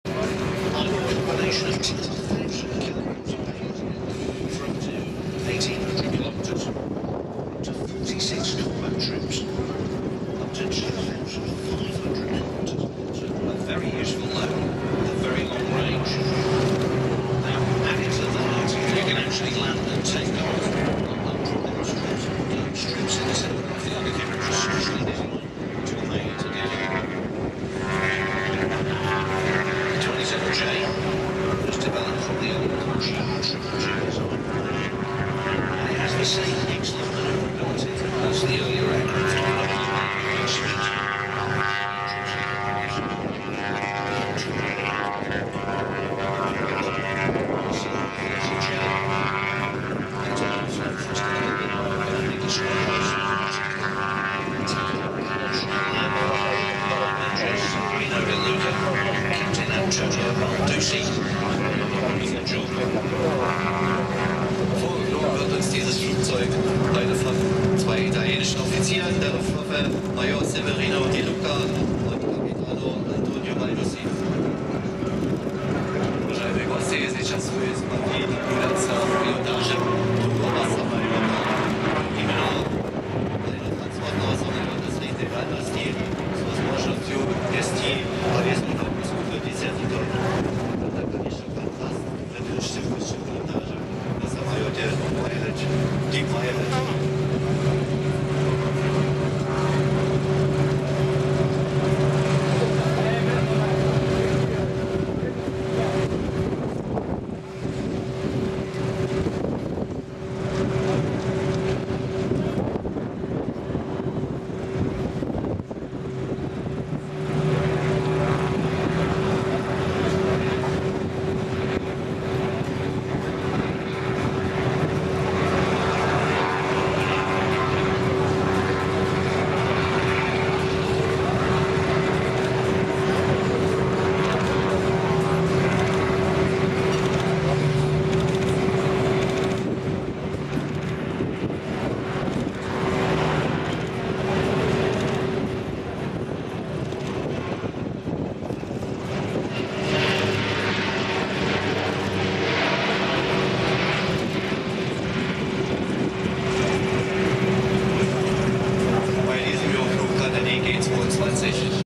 Alenia C-27J Spartan (ITAF)
Alenia C-27J Spartan of Italian Air Force at ILA 2010